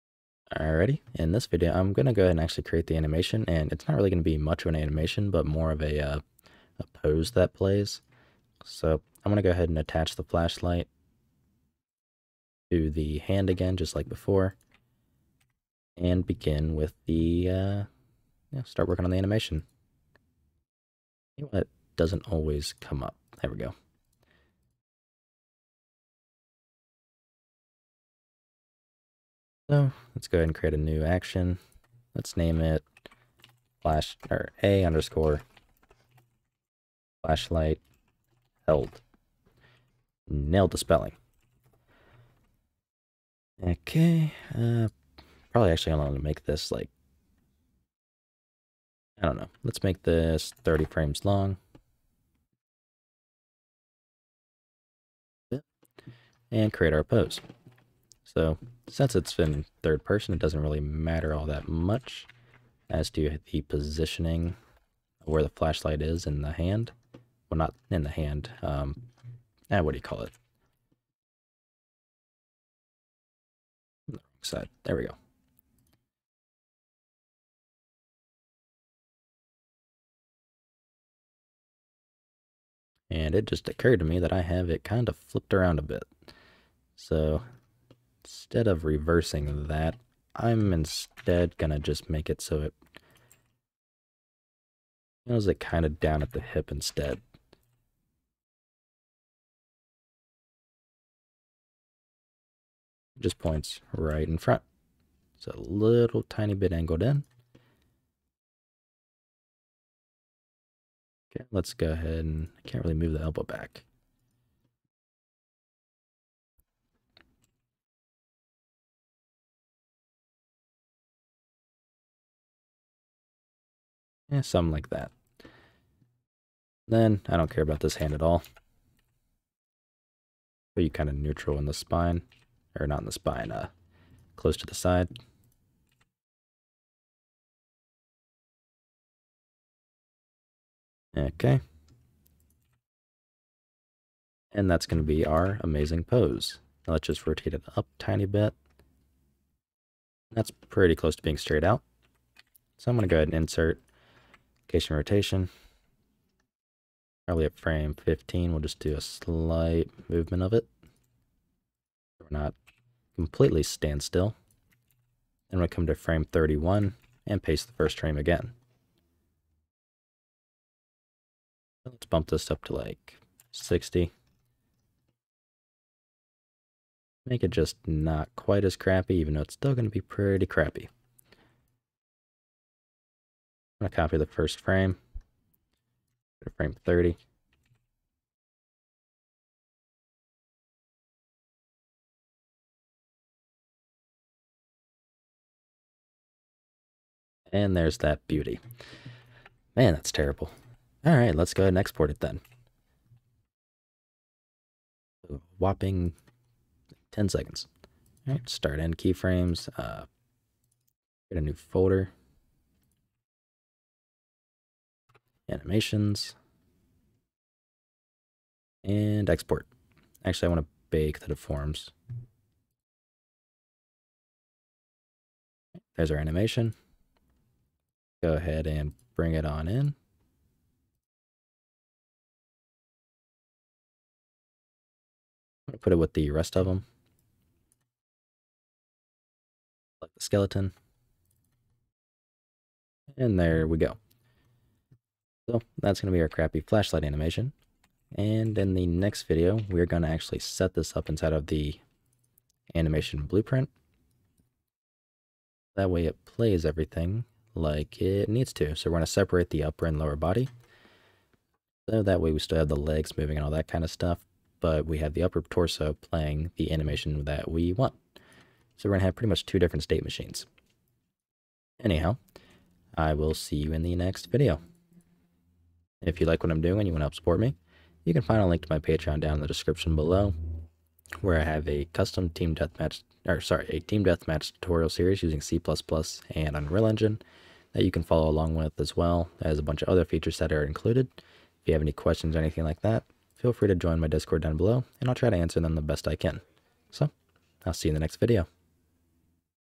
Unreal Engine C++ Multiplayer Flashlight #7: "Make Holding Flashlight Animation"
In this video we make a crappy animation for holding the flashlight to use to make it easier to aim our flashlight.